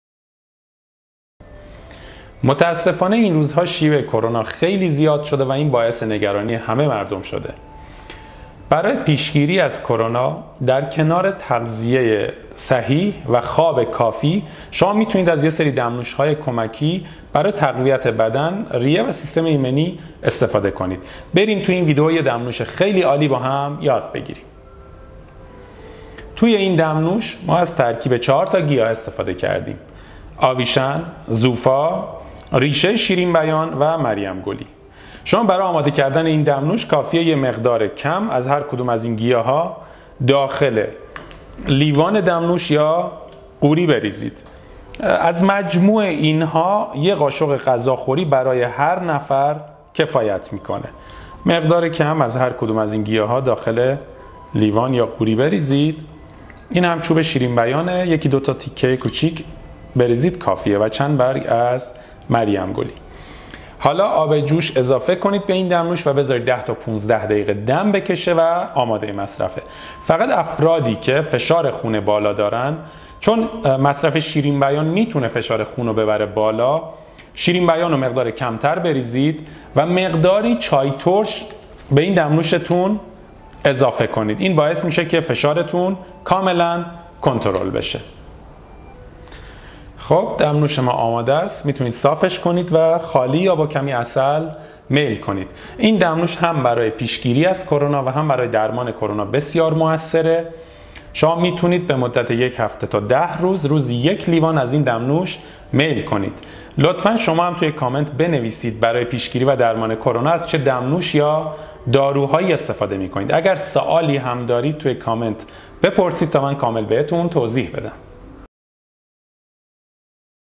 دمنوش تقویت سیستم و مفید برای کرونا
استفاده از دمنوش گیاهی یکی از راهکارهای عالی و بسیار موثر برای تقویت سیستم ایمنی بدن است که در برابر بیماری های ویروسی بسیار کارآمد است.
در این ویدئو یه دمنوش ساده و بسیار موثر برای تقویت سیستم ایمنی بدن معرفی کردیم.
برای دیدن ویدئوهای بیشتر و آموزش خواص و روش صحیح مصرف گیاهان دارویی و نسخه های رایگان لطفا روی لینک زیر کلیک کنید 👇👇👇👇👇